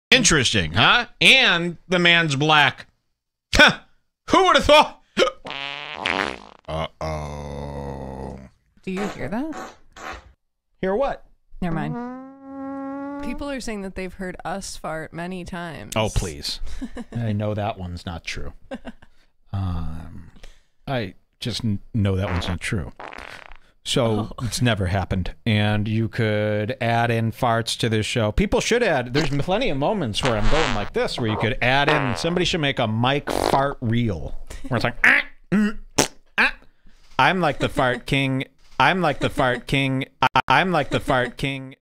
Mike Redbar Fart Reel
Real farts from Mike and Jules

A reupload of my most controversial film to date, since it got removed when Mike had my RBR Relay channel assassinated.